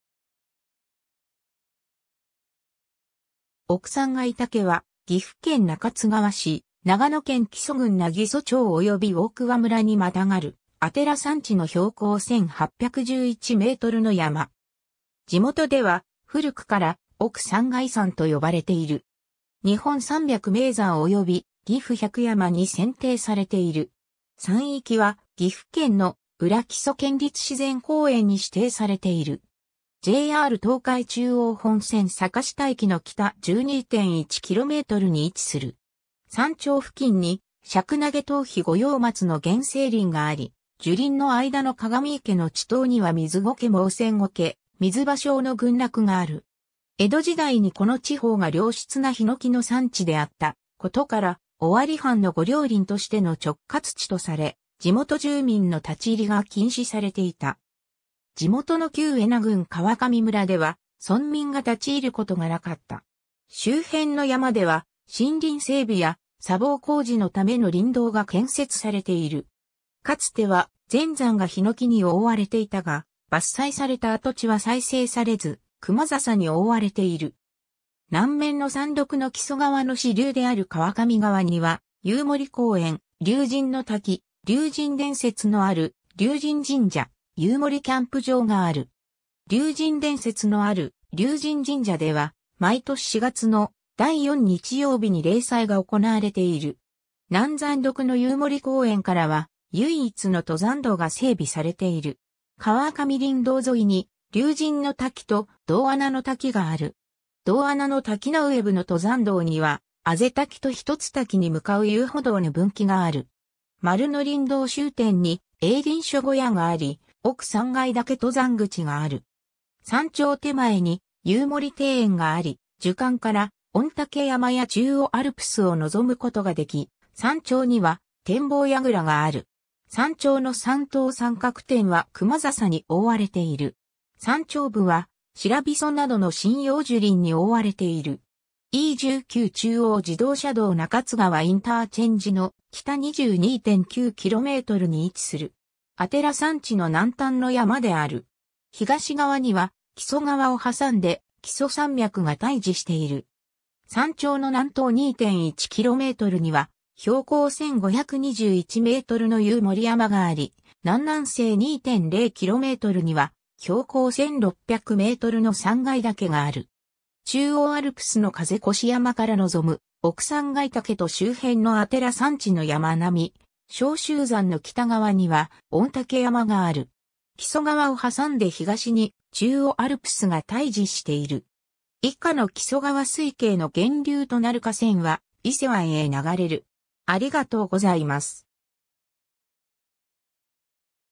奥三界岳
奥三界岳（おくさんがいだけ）は、岐阜県中津川市、長野県木曽郡南木曽町および大桑村にまたがる阿寺山地の標高1,811 mの山。
地元では古くから奥三界山（おくさんがいさん）と呼ばれている。
「日本三百名山」および「ぎふ百山」 に選定されている。
山域は岐阜県の「裏木曽県立自然公園」に指定されている。
 JR東海中央本線坂下駅の北12.1 kmに位置する。
山頂付近にシャクナゲ・トウヒ・ゴヨウマツの原生林があり、樹林の間の鏡池の池塘にはミズゴケ・モウセンゴケ・ミズバショウの群落がある。
江戸時代にこの地方が良質なヒノキの産地であったことから、尾張藩の御料林としての直轄地とされ地元住民の立ち入りが禁止されていた。
地元の旧恵那郡川上村では、村民が立ち入ることがなかった。
周辺の山では森林整備や砂防工事のための林道が建設されている。
かつては全山がヒノキに覆われていたが、伐採された跡地は再生されず、クマザサに覆われている。
南面の山麓の木曽川の支流である川上川には、夕森公園、竜神の滝、「竜神伝説」のある竜神神社、夕森キャンプ場がある。
「竜神伝説」のある竜神神社では、毎年4月の第4日曜日に例祭が行われている。
 南山麓の夕森公園からは、唯一の登山道が整備されている。
川上林道沿いに竜神の滝と銅穴の滝がある。
銅穴の滝上部の登山道には、アゼ滝と一ツ滝に向かう遊歩道の分岐がある。
丸野林道終点に営林署小屋があり、奥三界岳登山口がある。
山頂手前に夕森庭園があり樹間から御嶽山や中央アルプスを望むことができ、山頂には展望櫓がある。
山頂の三等三角点はクマザサに覆われている。
山頂部はシラビソなどの針葉樹林に覆われている。
 E19中央自動車道中津川ICの北22.9 kmに位置する。
 阿寺山地の南端の山である。
東側には木曽川を挟んで、木曽山脈（中央アルプス）が対峙している。
山頂の南東2.1 kmには標高1,521 mの夕森山があり、南南西2.0 kmには標高1,600 mの三界岳がある。
 中央アルプスの風越山から望む奥三界岳と周辺の阿寺山地の山並み、小秀山の北側には御嶽山がある。
木曽川を挟んで東（画像の手前側）に中央アルプスが対峙している。
 以下の木曽川水系の源流となる河川は、伊勢湾へ流れる。